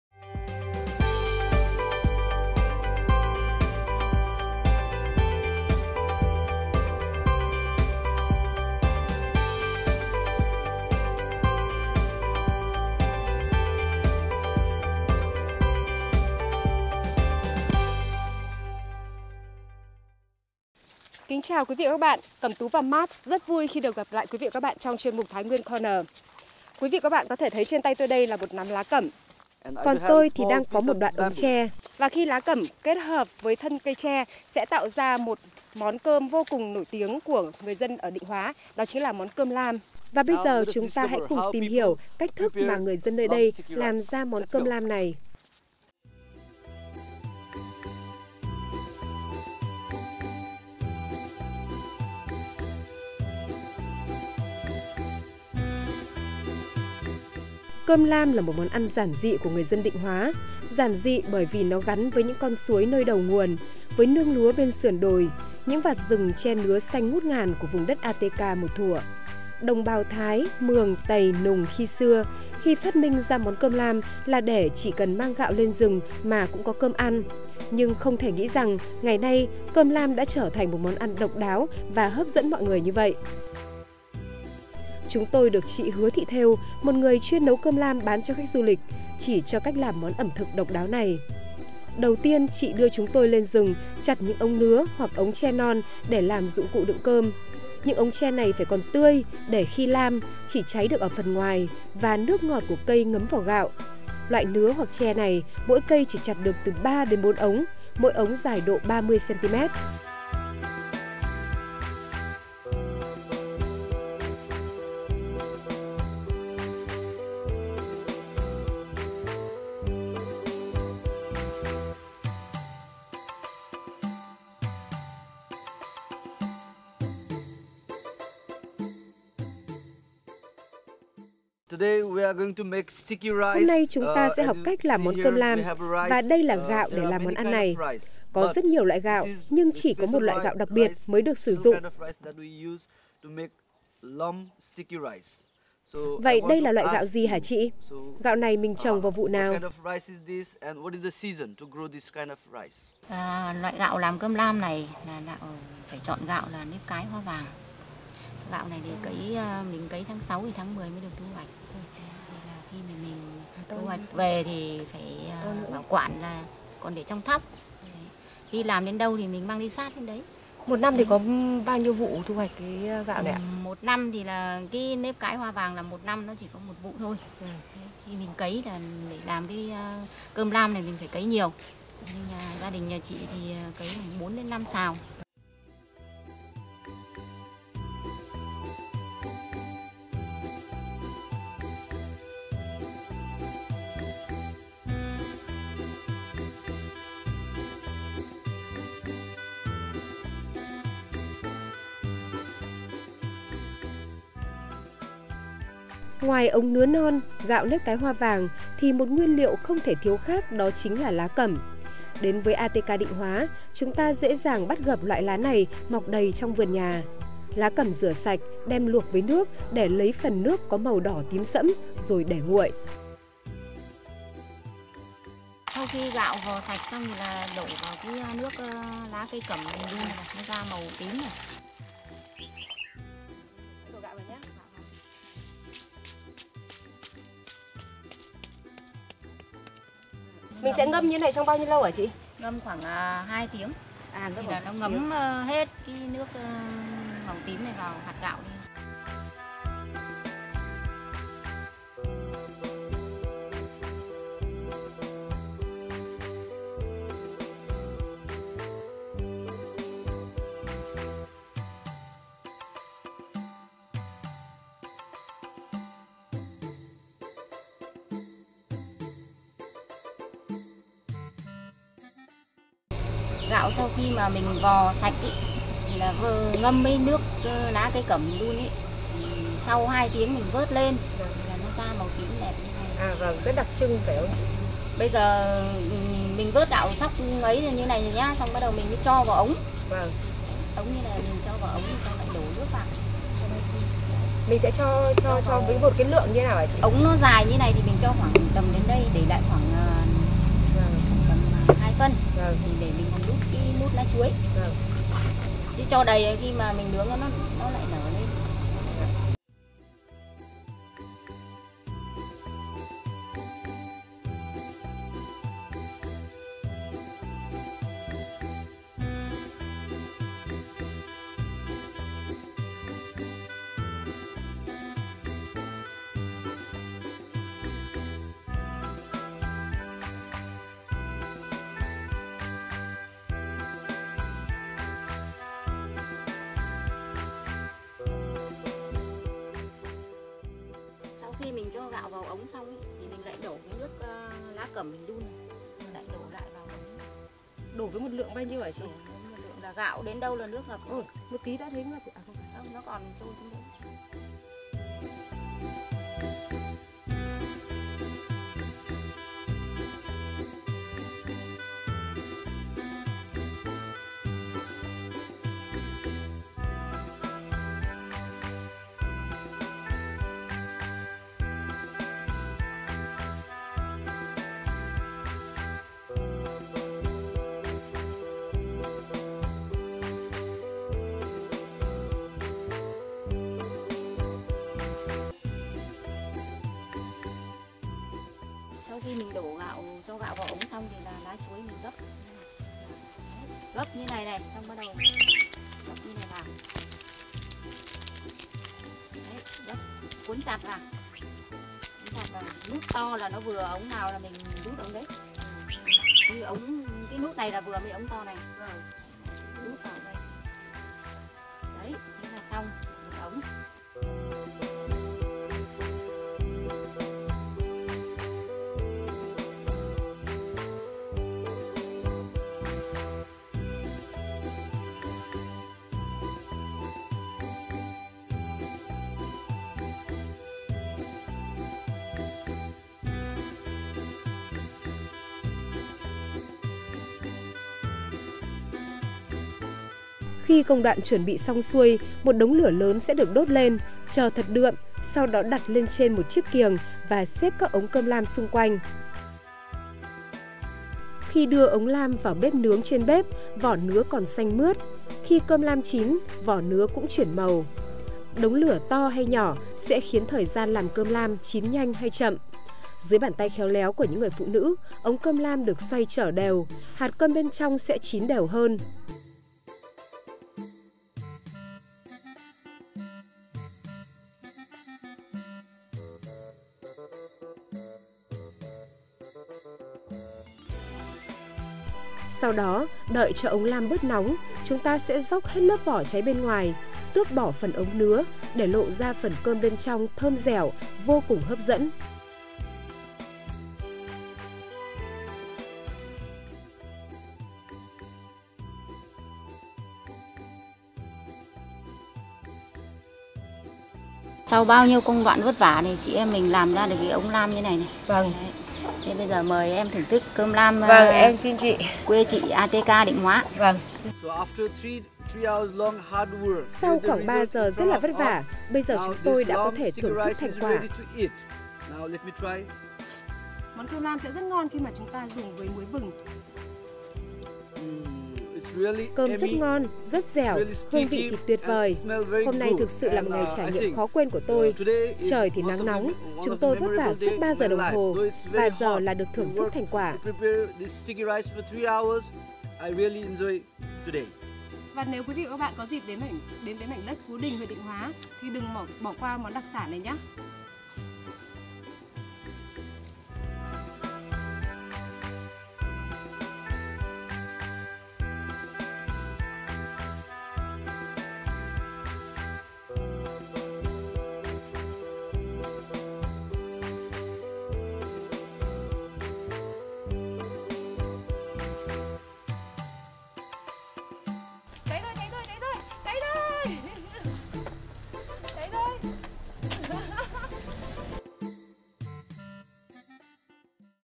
{太原角 2018} 竹筒饭 – 安全区的朴素食品
竹筒饭是定化县人民的朴素食品这里少数民族同胞发明竹筒饭是为了少数民族去耕作或者赶圩时随身带的饭食。只要带上少量米就有饭来吃。但是今年竹筒饭就成为精致和吸引力的食品。